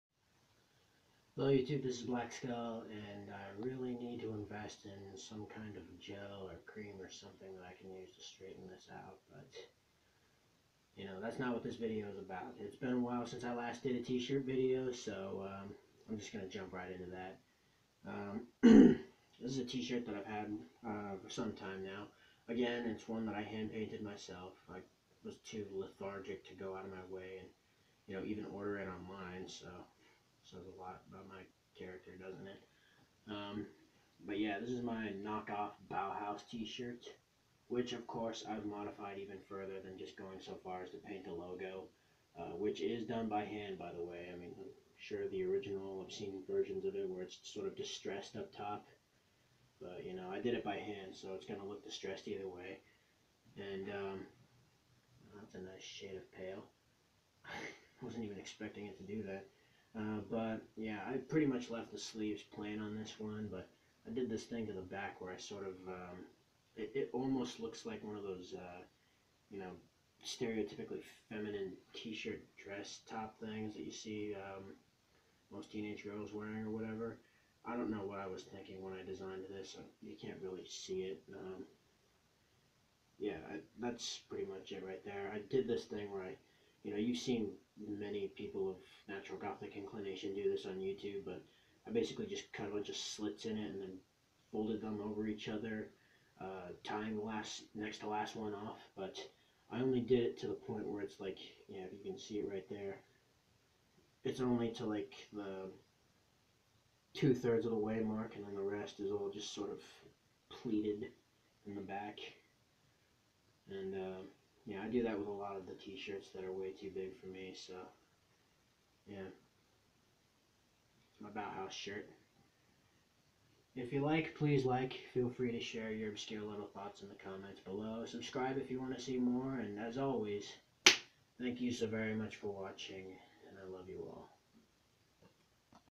DIY "Bauhaus" T-shirt
A hand painted t-shirt, with diy androgynous detail in the back, to better fit around the torso.